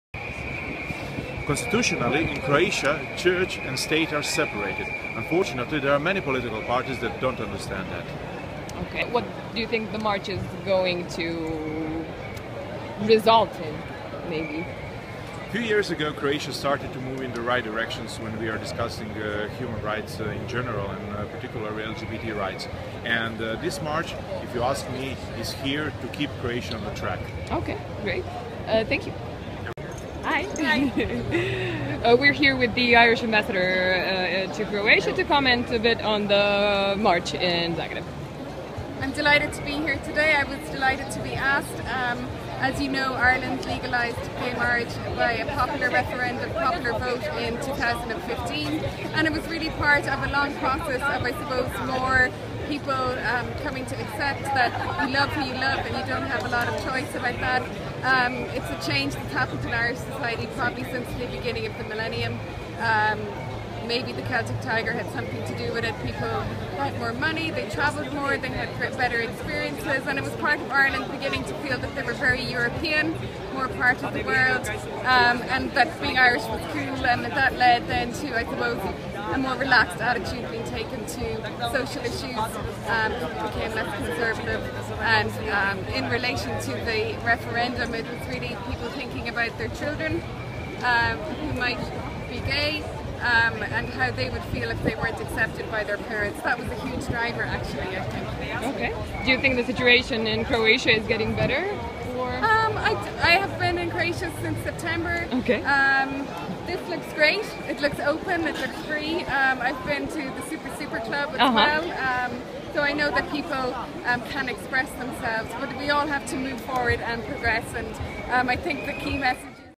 Zagreb Pride 2017 Comments
Pedja Grbin, Croatian lawyer and politician, and Mrs Olive Hempenstall, Irish Ambassador to Croatia, comment on Zagreb Pride 2017.